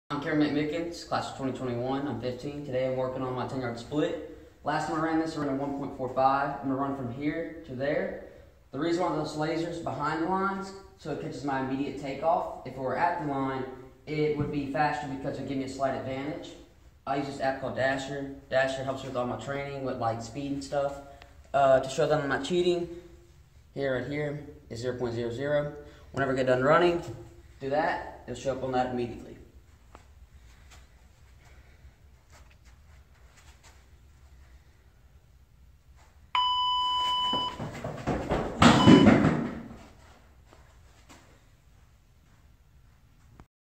Cameron McMickens, Class 2021, 15 years old, 1.40 ten yard dash laser time, Baseball Speed
Cameron McMickens, Class 2021, 15 years old, working on quickness and take off speed.  1.40 ten yard dash laser time. Gained 15 pounds of muscle in last 3 months since basketball is over and could concentrate on weight training and baseball